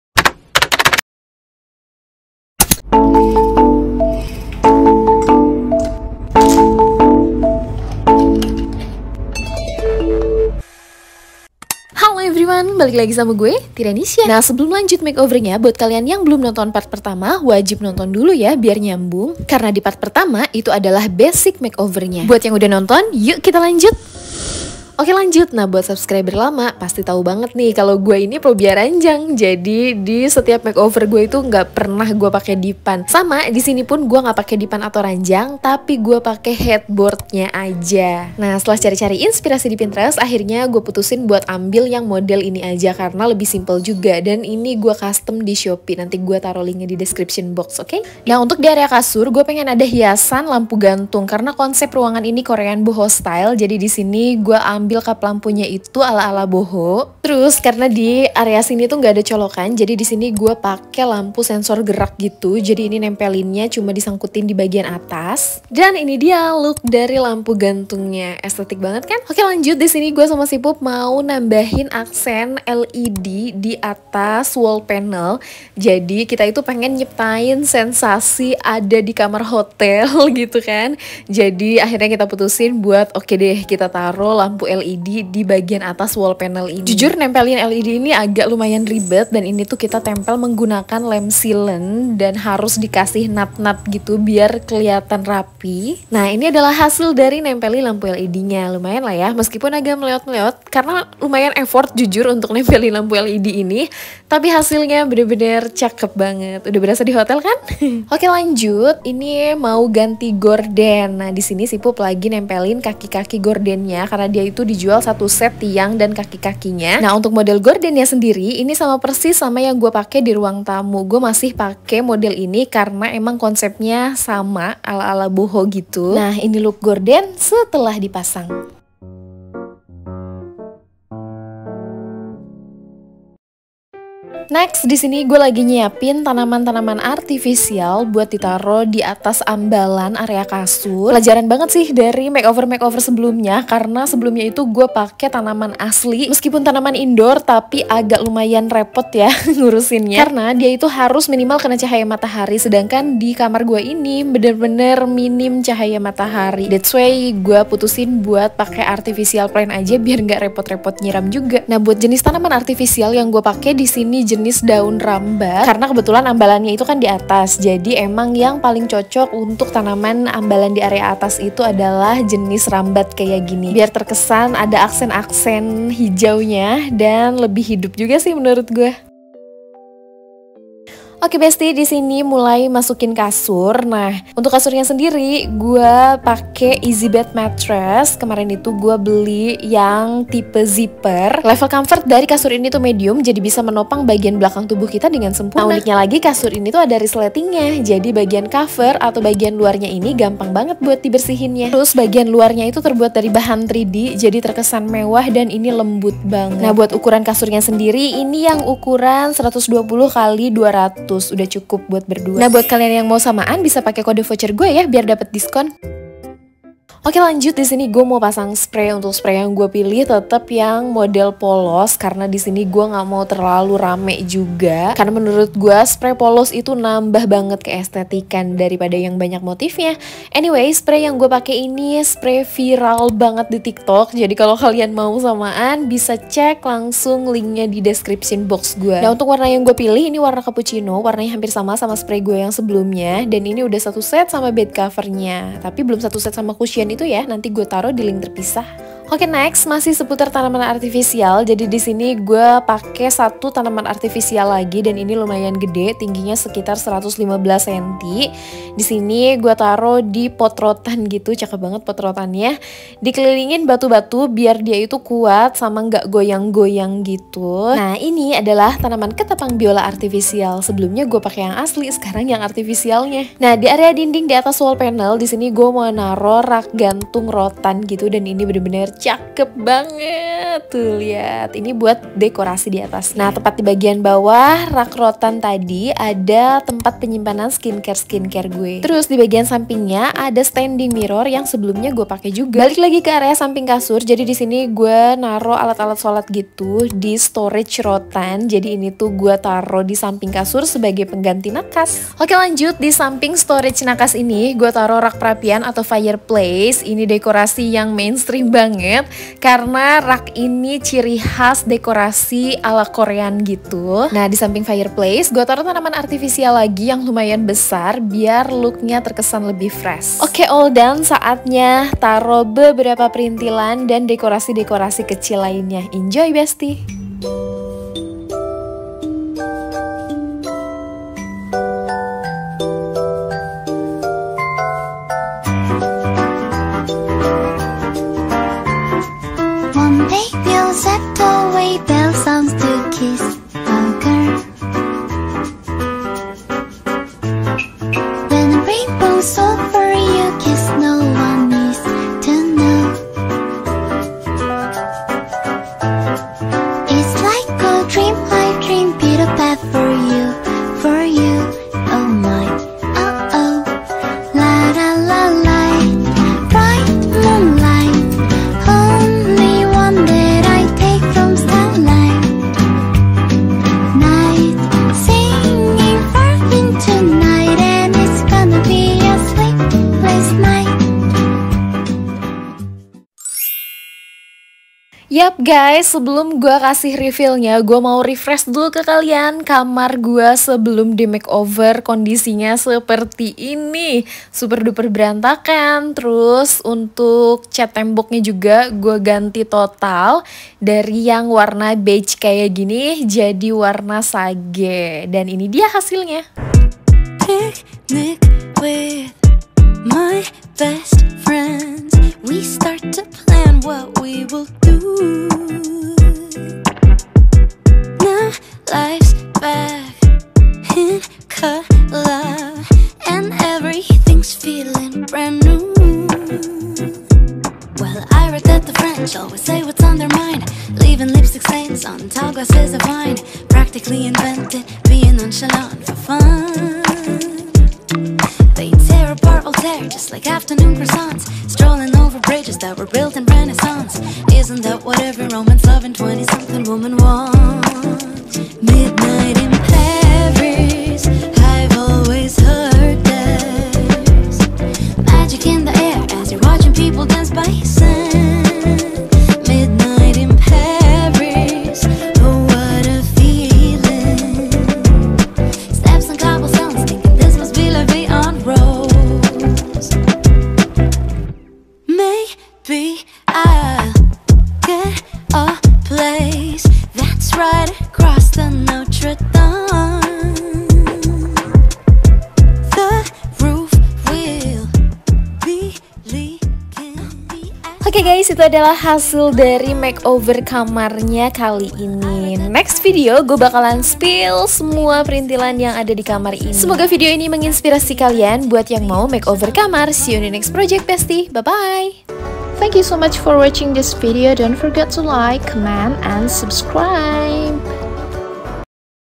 EXTREME BEDROOM MAKEOVER - Sage Korean Boho Style ala Pinterest | Kamar 3x4m Aesthetic on Budget (2)
Haiii Bestieee.. Finally Bedroom Makeover Part 2 sampai selesai niihh...

Berikut ini link produk dalam video ya bestie :
✨BASIC MAKEOVER :

Editing Video : Wondershare Filmora 9.5 + Inshoot
Editing Thumbnail : Lightroom + Photoshop CS5

I'm not own any music on my video.
By : BGM President, Anja Kotar.

XOXO!